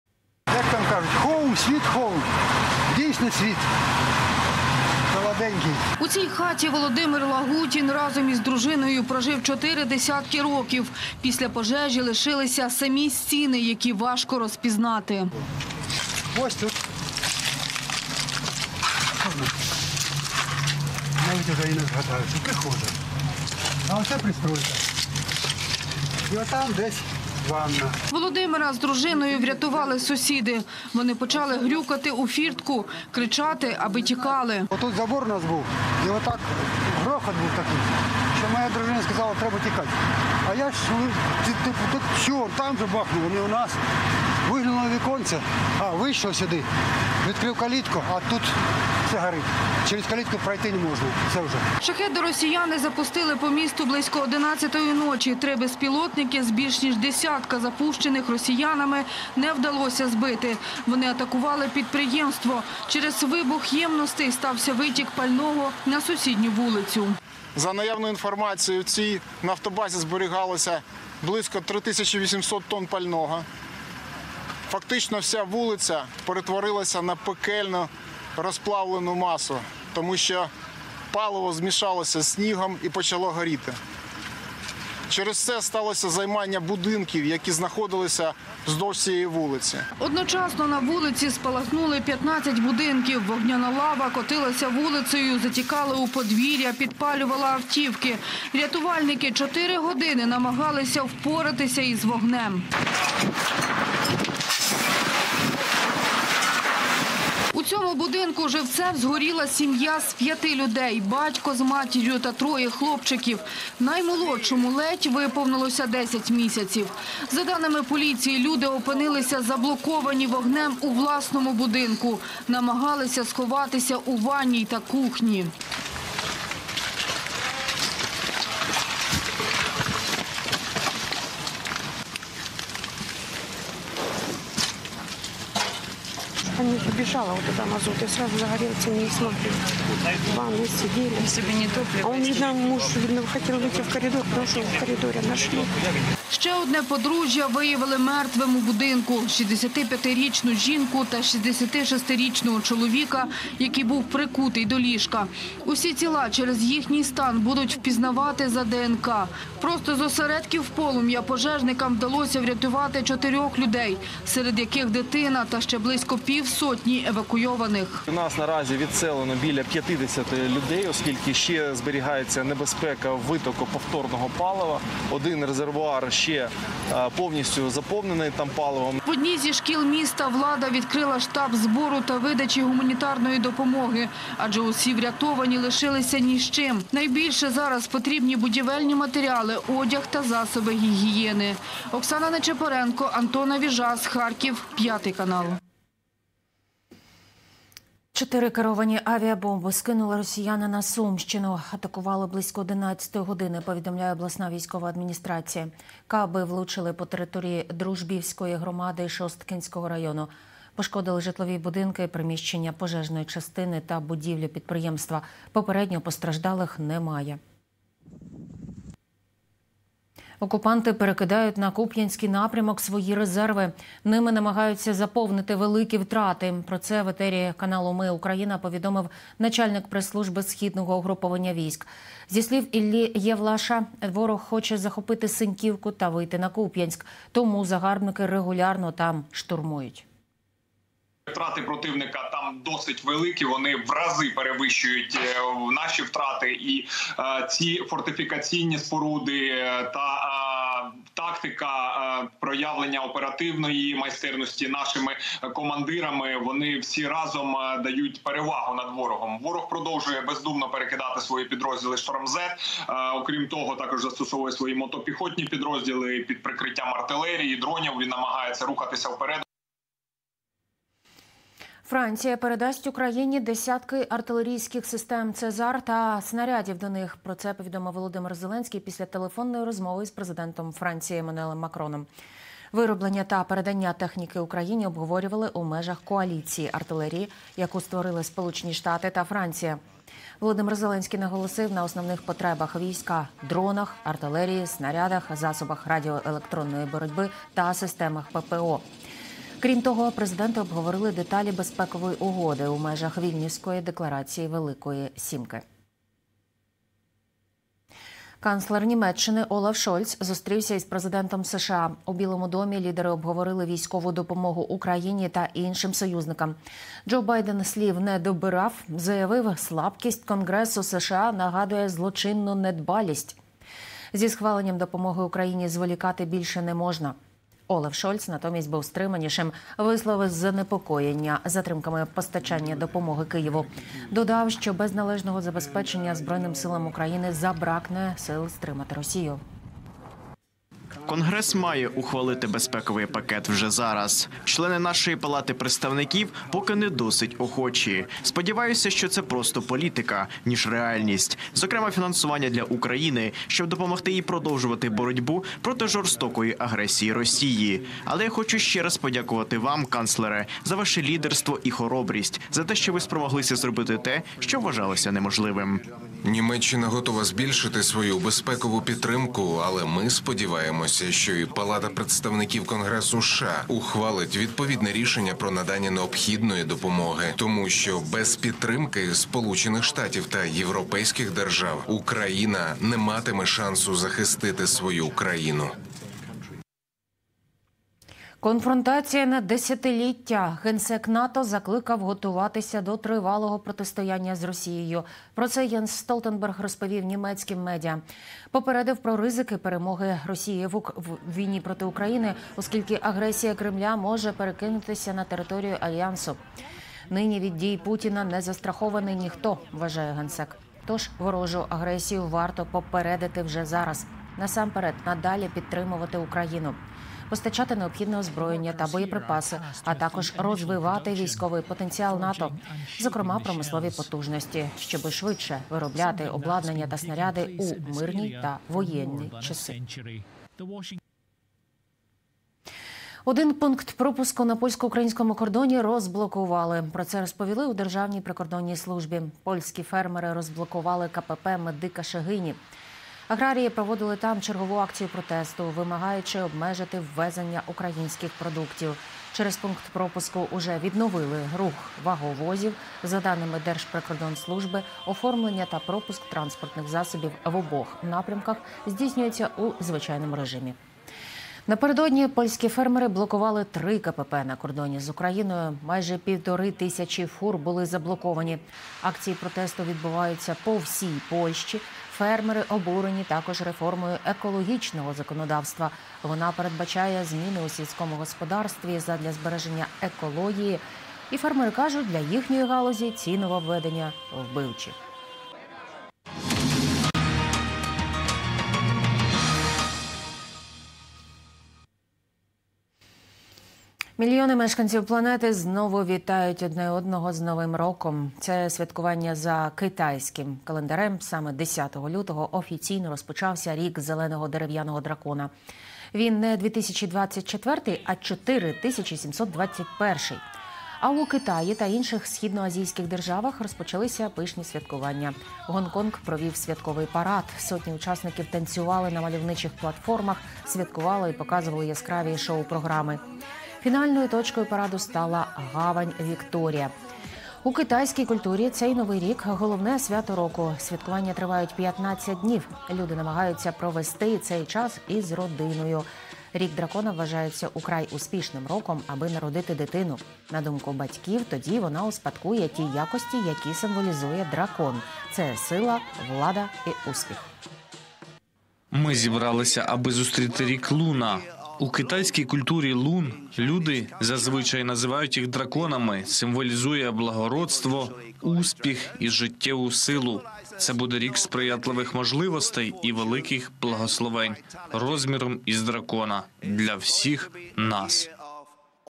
ВОГНЯНА ПАСТКА.💔7-ро загиблux у Харкові. 15 будинків спалахнули через ворожий удар
7-ро людей у Харкові загинули від удару російських Шахедів. Вони опинилися у вогняній пастці у власних будинках. Серед загиблих - троє дітей. Безпілотники поцілили у нафтобазу. Через вибух стався витік та загоряння пального, яке підпалило сусідню з підприємством вулицю. Майже одночасно там спалахнули 15 будинків. Оксана Нечепоренко продовжить.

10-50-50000 - будь-яка сума має значення.

Разом переможемо!
Слава Україні!